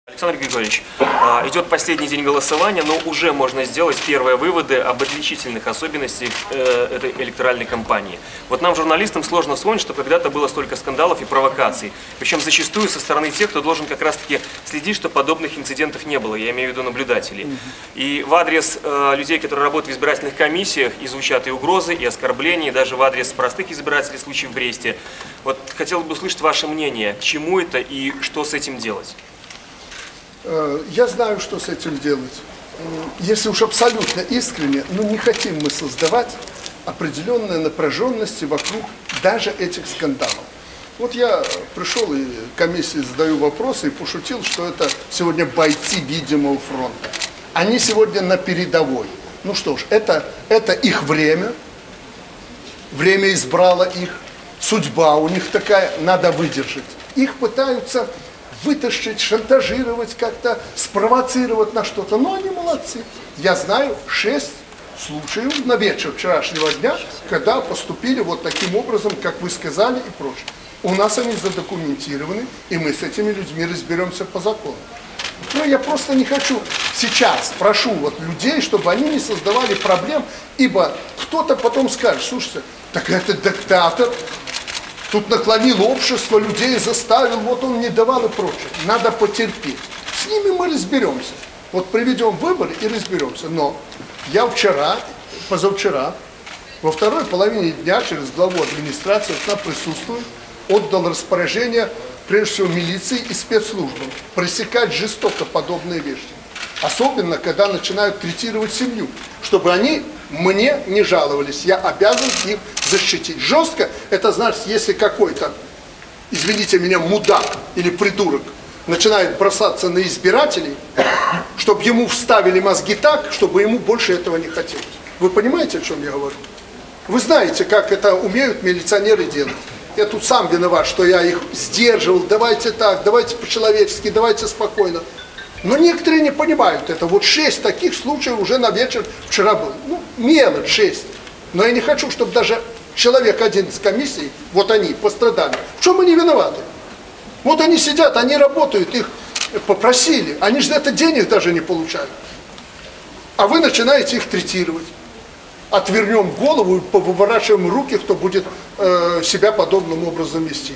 Лукашенко о провокациях на выборах
Мы не хотим создавать напряженности вокруг скандалов, которые произошли на избирательных участках. Об этом Президент Беларуси Александр Лукашенко заявил сегодня журналистам.